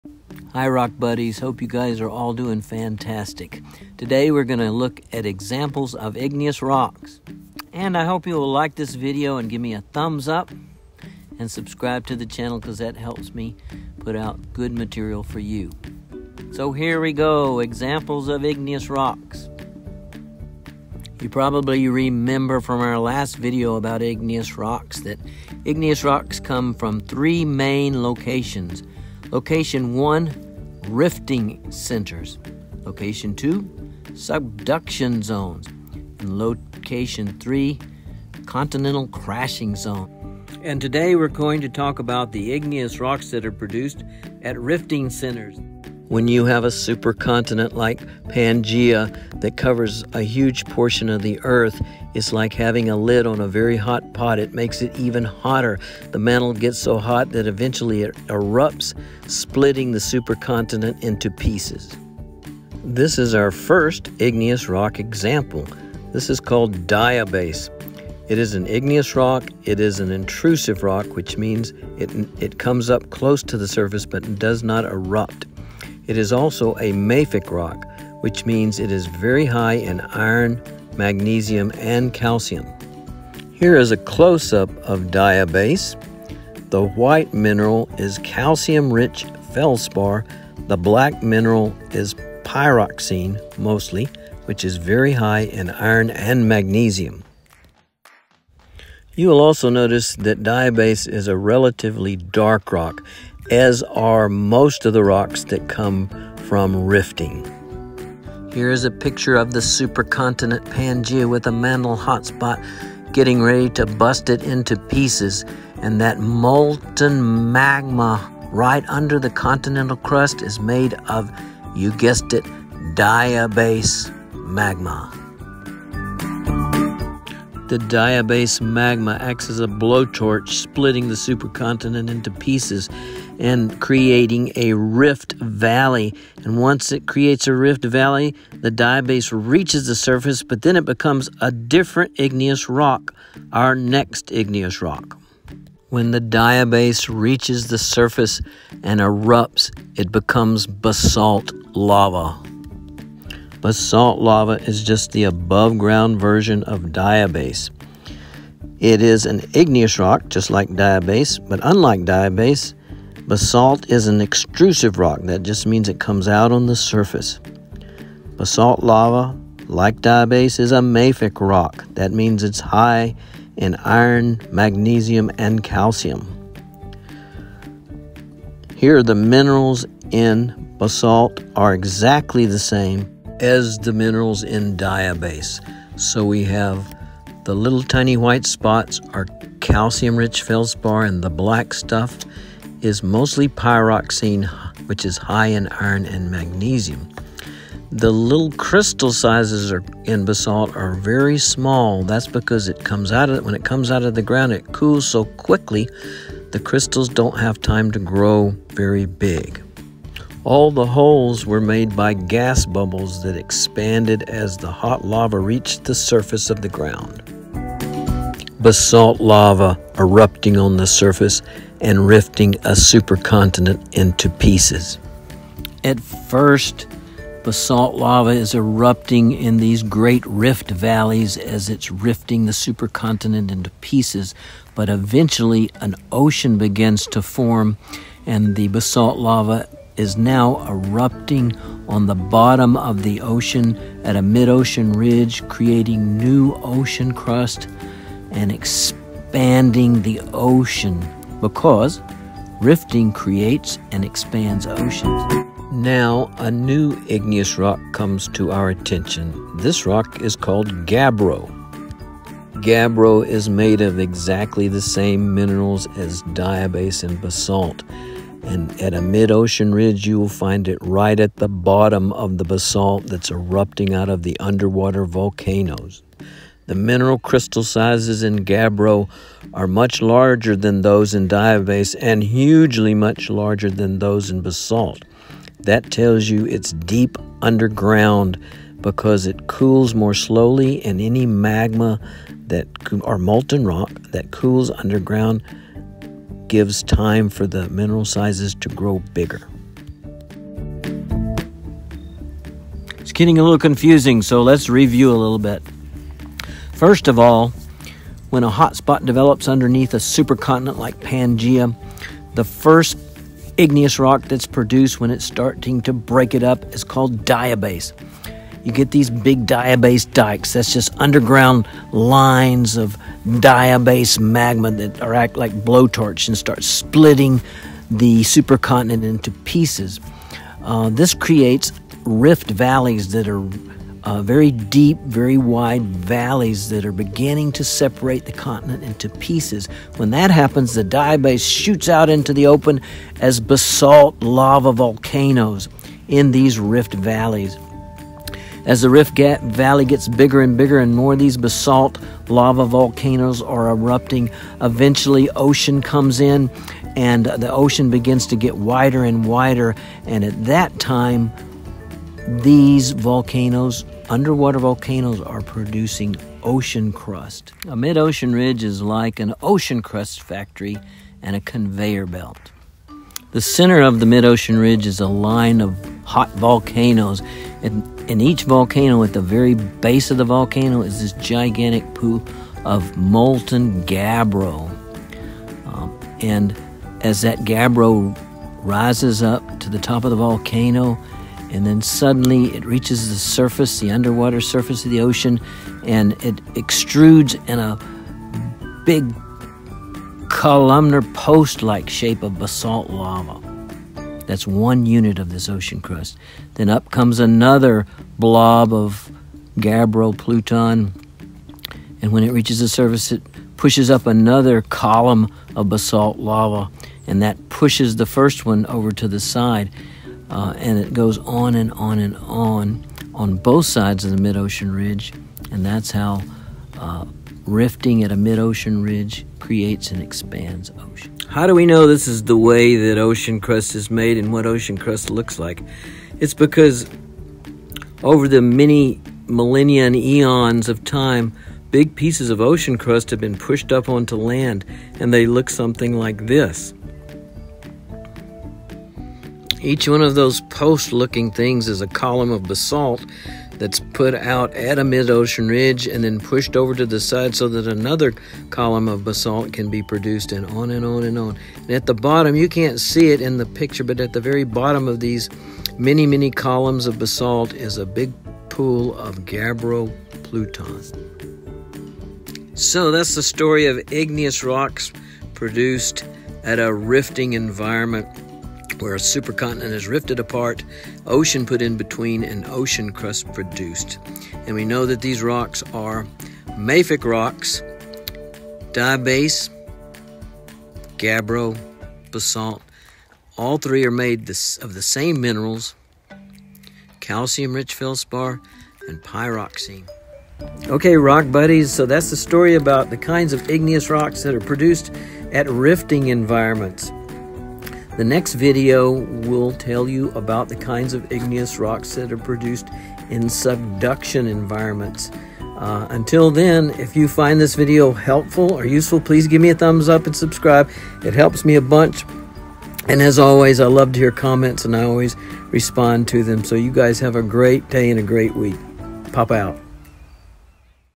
Igneous Rock Samples
This video talks about the kinds of igneous rocks that are found at rifting  centers.  This video also shows examples of igneous rocks found in rifting centers.￼A supercontinent like Pangaea covers a huge amount of the earth surface and it acts just like the lid on a hot pot of boiling water. it makes the pot boil quicker and hotter the same thing happens to the mantle underneath a supercontinent. The mantle begins to boil up and split the supercontinent into pieces this is called rifting.￼ The first kind of igneous rock produced as the upwelling boiling mantle begins to split a supercontinent into pieces is called DIABASE. DIABASE is a mafic igneous rock. Mafic means the rock is high in iron magnesium and calcium.￼  May fake rocks are dark in color and heavy in weight. The minerals found in DIABASE are calcium rich Feldspar and Pyroxene.￼. DIABASE is also an intrusive igneous rock which means that it never reaches the surface of the ground or the ocean bottom.  Hot molten DIABASE rises up from the mantle and acts as a blow torch to split the supercontinent into pieces. But when the DIABASE reaches the surface and comes out onto the surface it is a new rock called basalt lava.￼  A wide rift valley has been created by the DIABASE in it gets filled with large volcanoes that irrupt masses of basalt lava.￼  Basalt lava is exactly the same mineral logically as DIABASE it is just diabase that has irrupt it onto the surface.￼  As more basalt volcanoes irrupt the Rift Valley gets larger an ocean floods in to begin the creation of an ocean.￼ Rifting creates an expands oceans.￼  ￼ as the rifting process continues the ocean widens and becomes a mid ocean ridge rifting site.  At the mid ocean ridge ocean crust is being produced on either side of the main Ridge.￼   Ocean crust is produced at the mid ocean ridge when a large pool of gabbro pluton irrupt a column of basalt lava onto the Ocean floor.￼  Gabbro has exactly the same minerals is basalt and DIABASE but it forms much deeper below the surface just at the top of the mantle.￼ Gabbro has bigger mineral crystals then either DIABASE or basalt because it was formed deep are under the surface and so cool more slowly. When a magma like gabbro cools slowly crystals have time to grow large.￼  Basalt lava has very tiny mineral crystals because it comes out onto the surface and cools very quickly in either the air or the water.￼  Ocean crust is made of mini long columns of basalt lava extruded side-by-side on both sides of the mid ocean ridge.￼ A large pool of gabbro is below each of these columns and supplies them with irruptive material.￼  We know this is how ocean crust is made because in the mini eons of time pieces of ocean crust have been shoved up onto land where we can visually see them.￼ A bunch of these basalt columns is called a she did Dyke complex and you can see examples of this at the devils Postpile national monument in California and at the Giants Causeway in eastern Ireland.￼￼￼